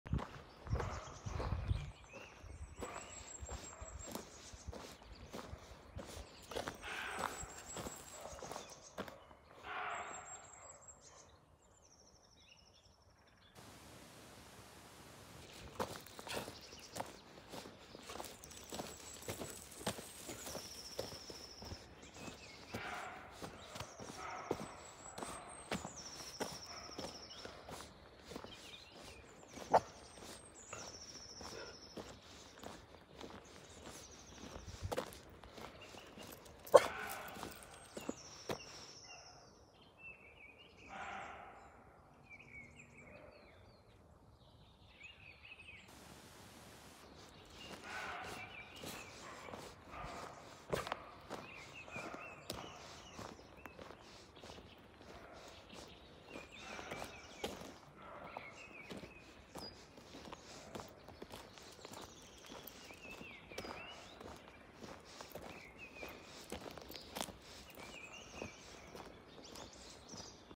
Did me and my dog just stumbled on BIGFOOT? Strange sounds in the forest, HELP!
I walk my dog everyday 2 - 3 hours in the woods, and I never heard a voice like this before.
Me and my dog heard a very loud shouting, and a reply to this shouting from another direction. Right after the shouting, my phone is making a white noise.
Is this all a coincidence, or can you guys tell me which animals are makes this noice?
Was a little scary,  thank you 💜